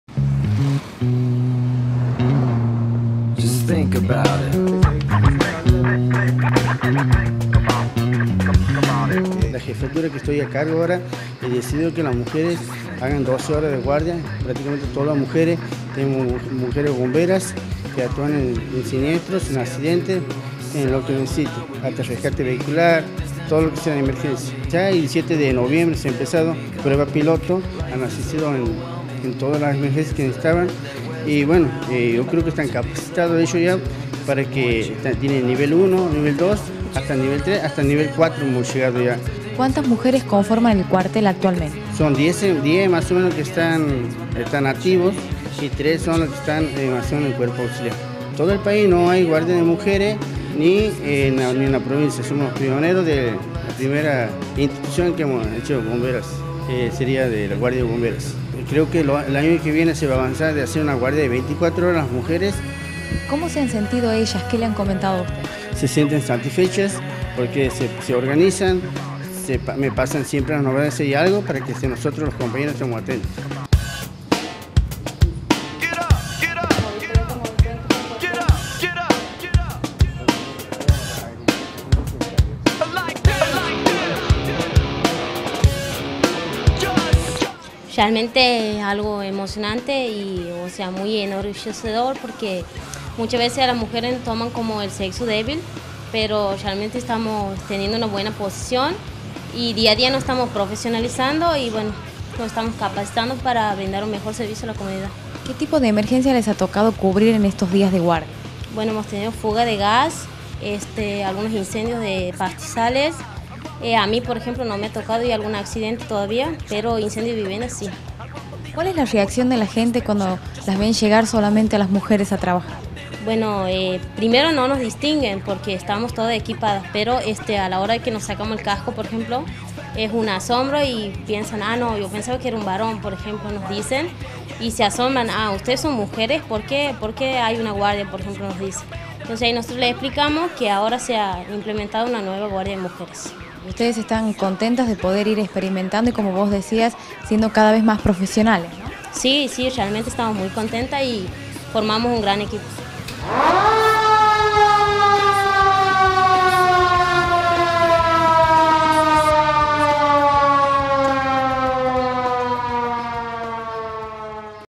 Bomberos voluntarios de La Banda pioneros en el país al implementar guardias femeninas
Cumplen guardias de 12 horas. Contaron su experiencia.